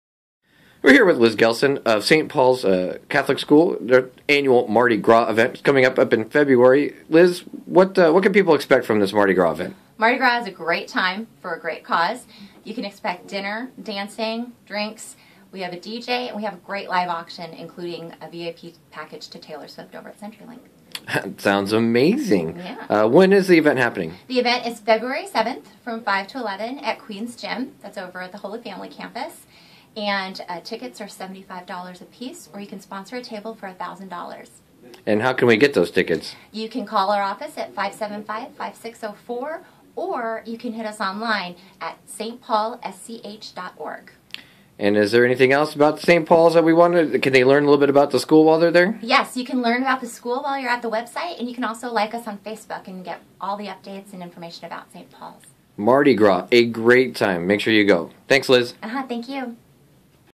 Liz Gehlsen Previews the St Paul Mardi Gras celebration
The annual Mardi Gras party at St. Paul's Catholic School will be happening in early February. Make sure you get hooked up with tickets.